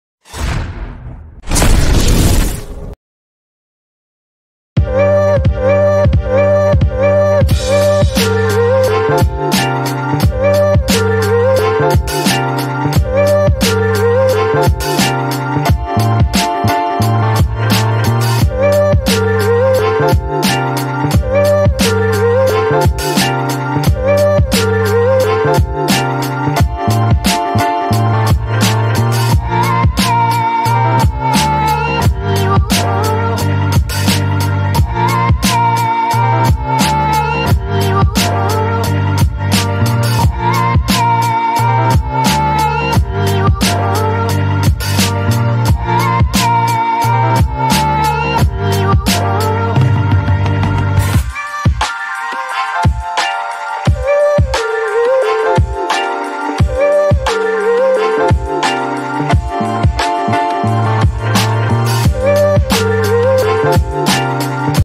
Redmi A2 India launch date confirm 🤩| Rumour & Spec leaked 🤩| 5000MAH battery 😍🤩|by technical akash
Hello Guys,
friends,is video me hum baat karenge Redmi A2 ke India launch date & Rumour & spec leaked ke baare me.
Redmi A2 comes with 5000MAH battery & Meditatek Helio soc .